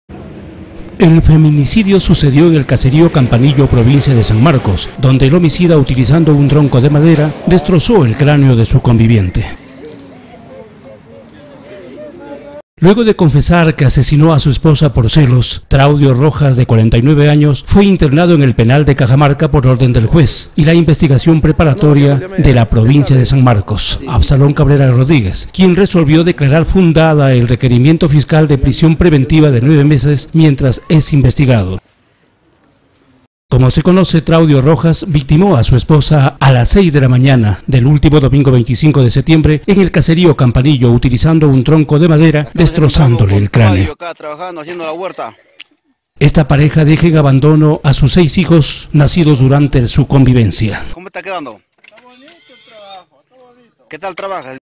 Ya está en la cárcel sujeto que mato a su esposa
El feminicidio sucedió en el Caserío Campanillo – provincia de San Marcos, donde el homicida utilizando un tronco de madera destrozó el cráneo de su conviviente.
Luego de confesar que asesinó a su esposa por celos, Truadio Rojas de 49 años, fue internado en el penal de Cajamarca, por orden del juez de investigación preparatoria de la provincia de San Marcos, Absalón cabrera rodríguez, quien resolvió declarar fundada el requerimiento fiscal de prisión preventiva de 9 meses, mientras es investigado.
La audiencia en su contra se realizó ayer en el juzgado de paz letrado con adición a sus funciones de juzgado de investigación preparatoria, que se ubica en el distrito de Pedro Gálvez, posteriormente el asesino confeso fue trasladado por personal de la policía judicial al penal de Cajamarca.
Como se conoce Truadio rojas victimó a su esposa a las seis de la mañana del último domingo 25 de setiembre en el caserío campanillo, utilizando un tronco de madera, especie de leña, destrozándole el cráneo.
Esta pareja deja en el abandono a sus 6 hijos nacidos durante la convivencia.